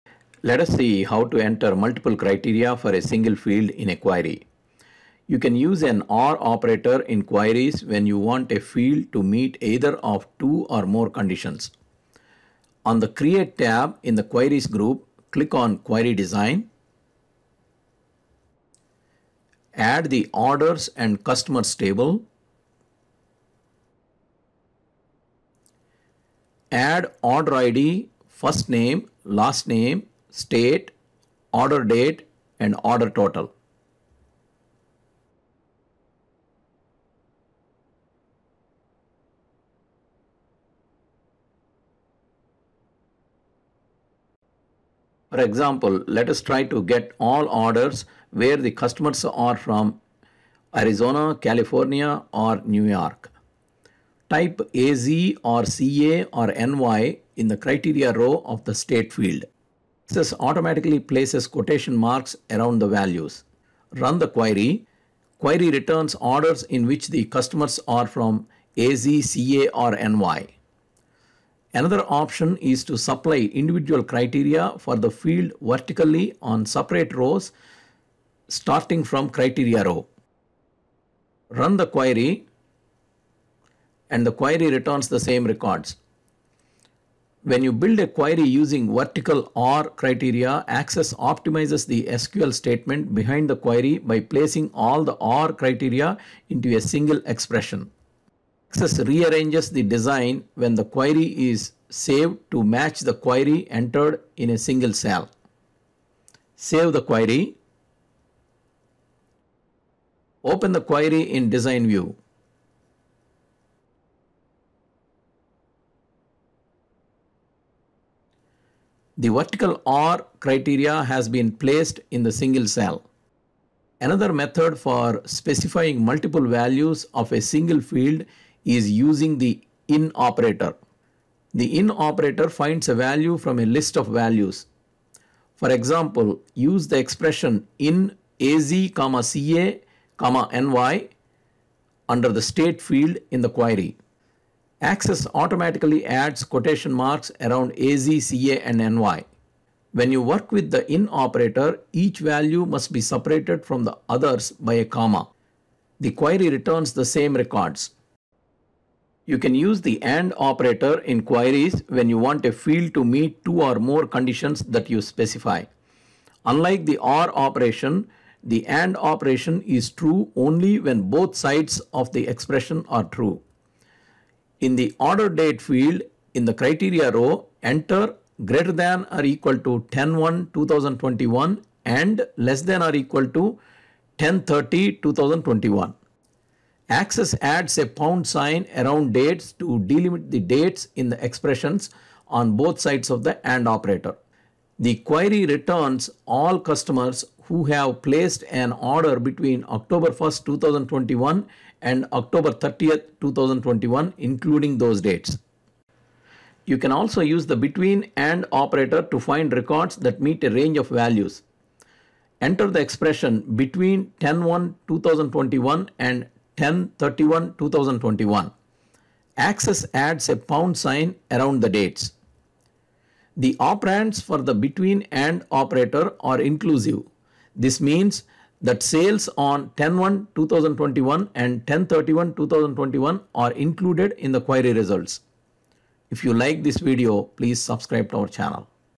Multiple Criteria for a Single Field in MS Access - Office 365
You can enter multiple criteria for a single field in query. You can use OR operator in queries when you want a field to meet either of two or more conditions. Another method for specifying multiple values of a single field is using the "IN" operator. The "IN" operator finds the value from a list of values. You can also use the AND operator with greater than and less than operators to find a range of values. You can use BETWEEN, AND operators also to find the range of values. This video explains in detail the different ways you can use multiple criteria for a single field.